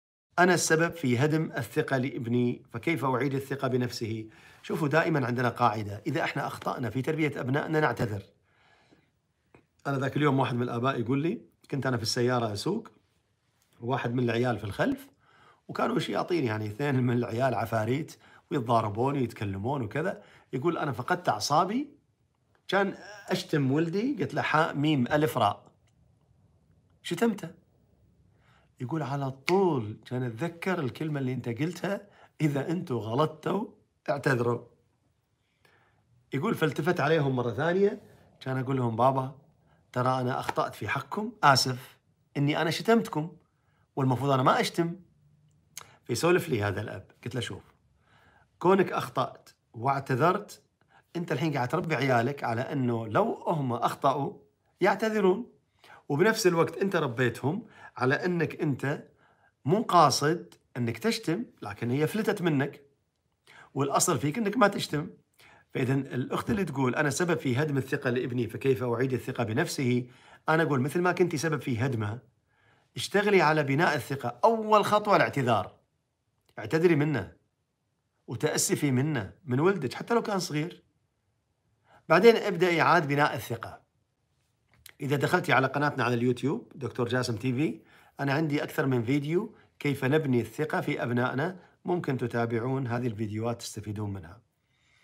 هدمت ثقة طفلي بنفسه كيف اعيد بناءها ؟
هل هناك حل لمن هدم ثقة طفله بنفسه ؟؟
موقع دكتور خطوبة خاص للمقبلين على الزواج والمتزوجين حديثا
موقع خدمة معين التربوية الالكترونية الخاصة بالأطفال من عمر 2 الى 15 سنة